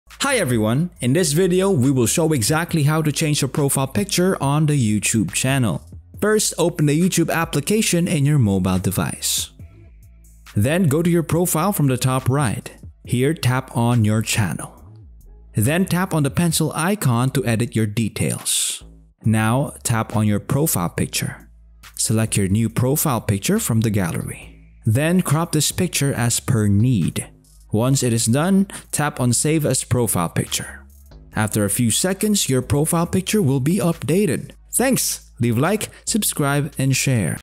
How To Change Profile Pic In Youtube Channel
In this video I will show you How To Change Profile Pic In Youtube Channel